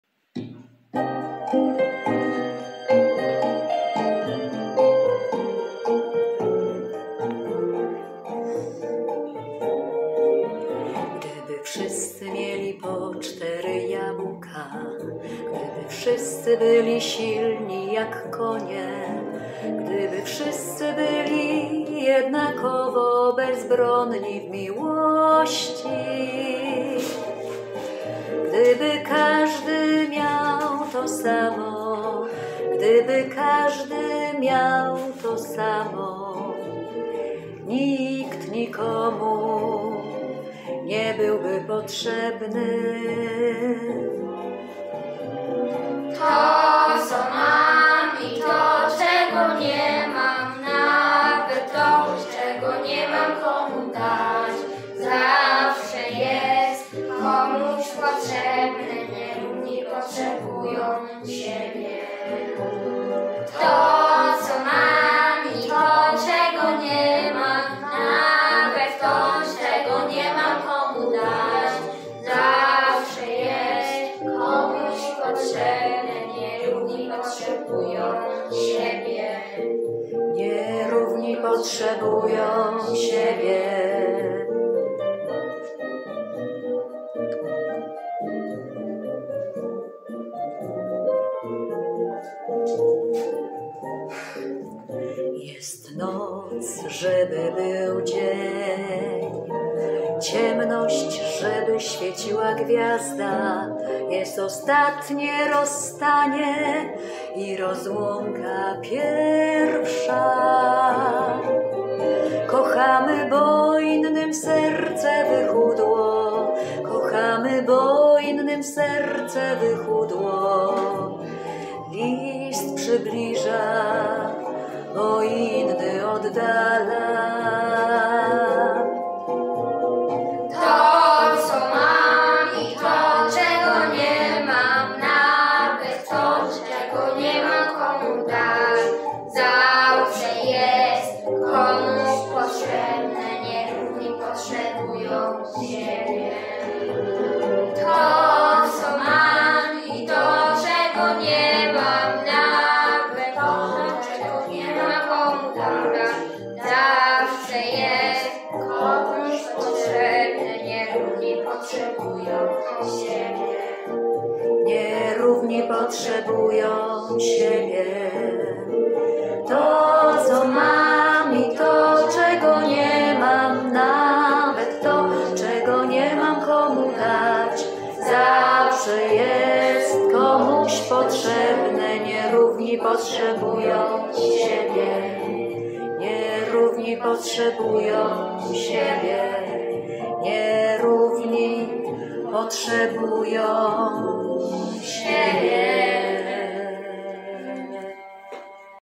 Piosenka ze słowami ks. Jana Twardowskiego
ZSS w Kowanówku, lekcja muzyki w klasie łączonej  7A.